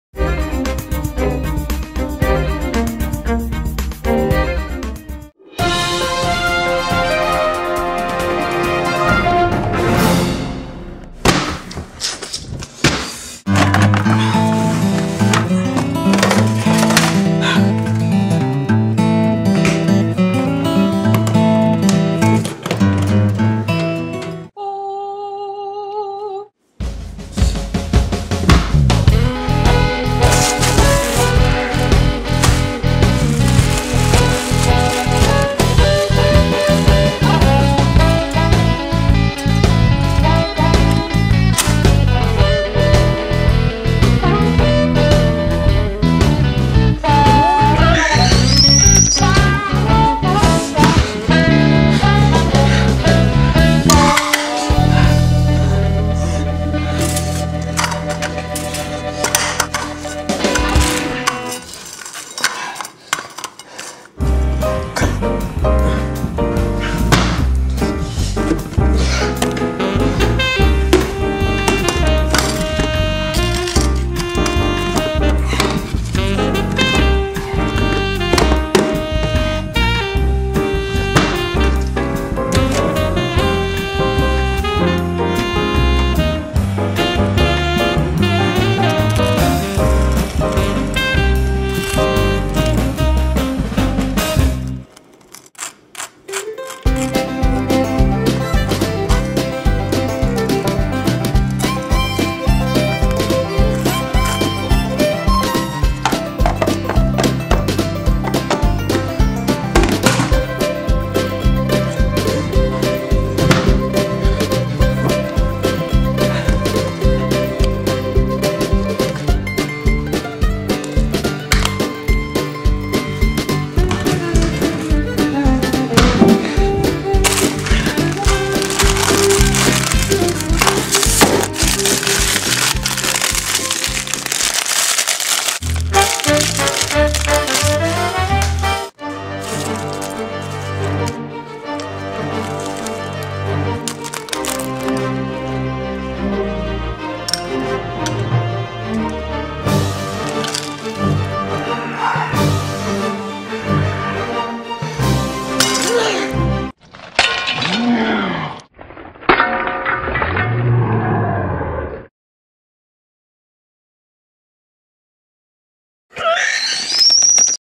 ~Toast~
In this exiting episode of Cakepig01 skits. I make toast.